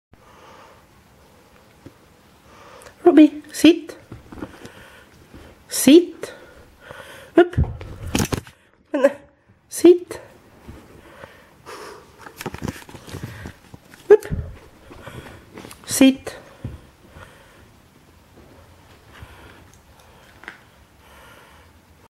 Hyper Ruby
Trying to make a video of my cavalier king charles puppy doing some tricks,but it faild XD